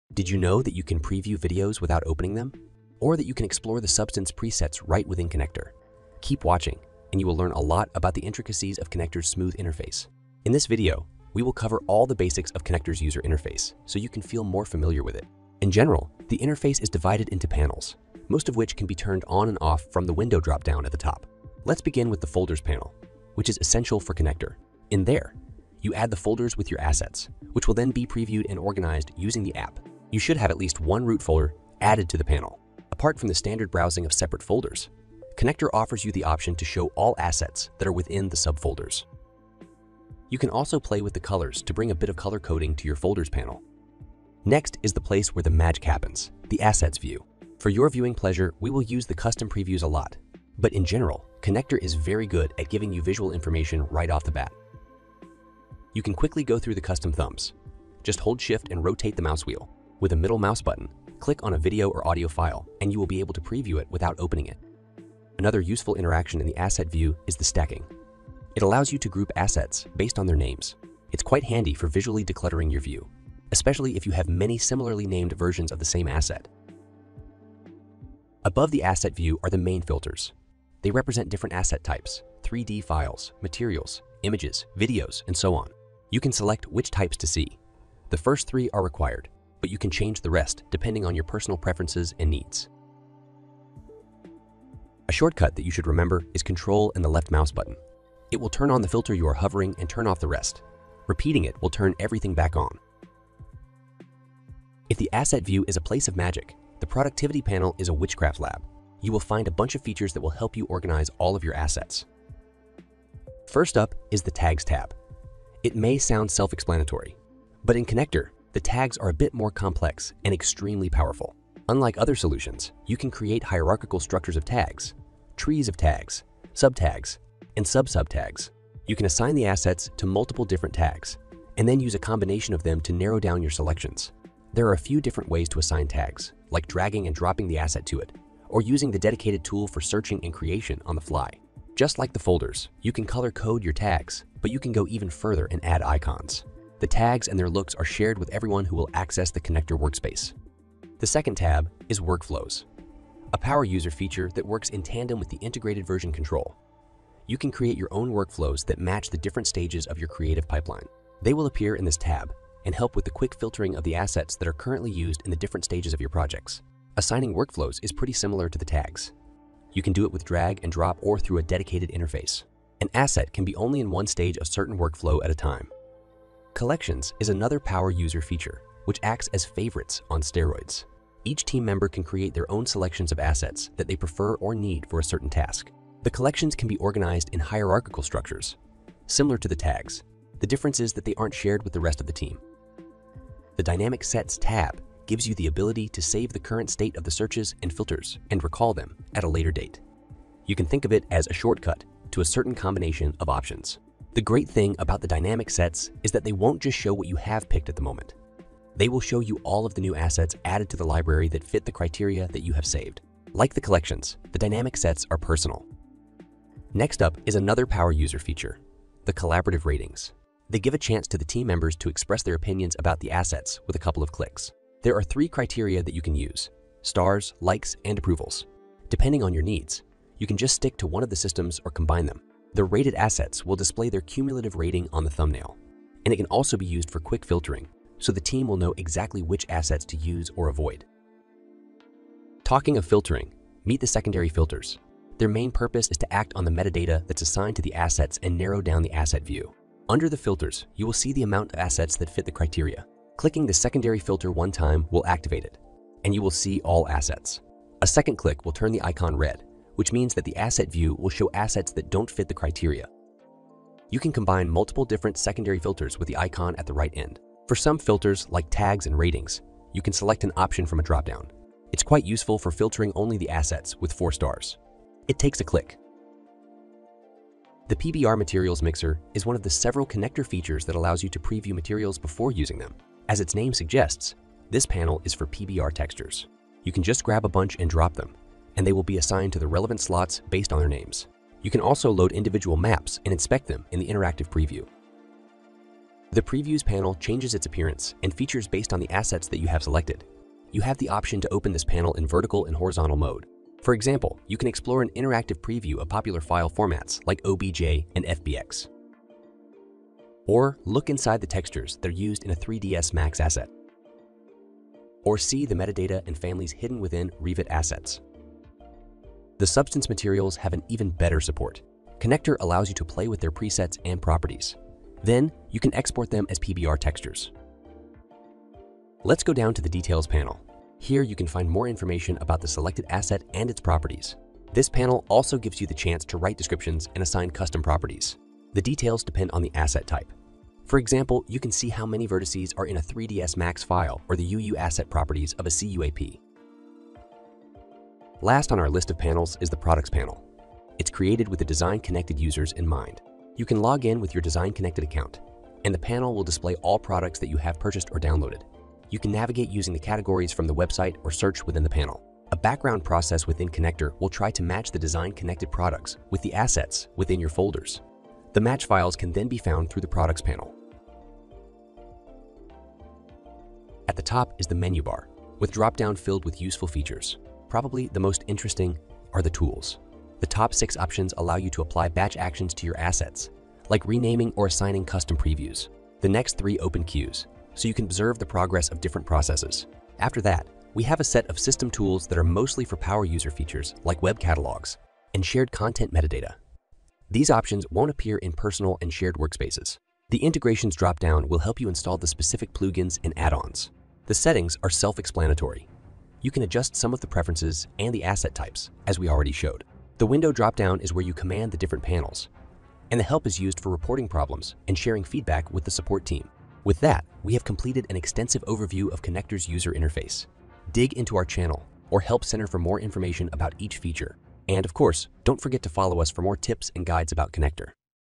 Deep dive in Connecter's UI
Connecter's interface may look minimalistic and smooth, but it gives you access to tons of powerful features. Some of them - a bit hidden. Explore it in this long tutorial that covers it all.

00:00 - Intro
00:24 - Folders Panel
00:45 - Assets View
01:18 - Main Filters
01:44 - Productivity Panel
01:54 - Tags Tab
02:35 - Workflows Tab
02:58 - Collections Tab
03:14 - Dynamic Sets Tab
03:37 - Ratings Tab
04:02 - Secondary Filters
04:36 - PBR Material Mixer Panel
04:56 - Previews Panel
05:27 - Details Panel
05:47 - Products Panel
06:11 - Menu Bar
06:57 - Outro

The voiceover is created with Play.HT.